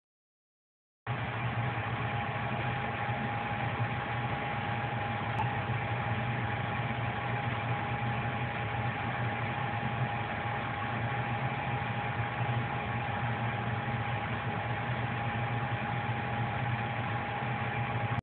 Man gets owned by gate at impound yard
customers at impound yard after they returned from a bus trip to find there cars had been towed.